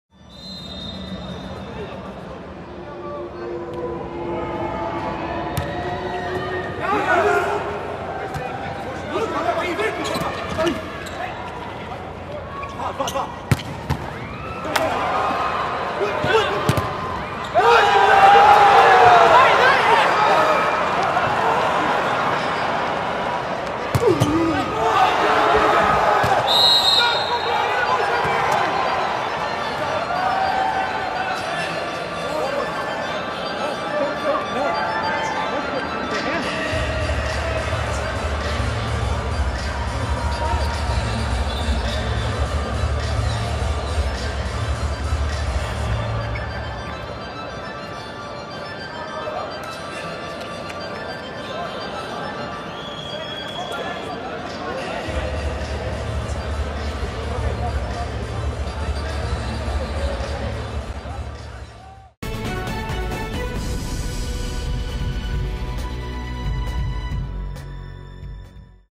World League Final 6: Magical dig from Massari!
Jacopo Massari pulls off a magical dig for Italy against Serbia in the second match of the World League Final 6 in Rio.

Join the conversation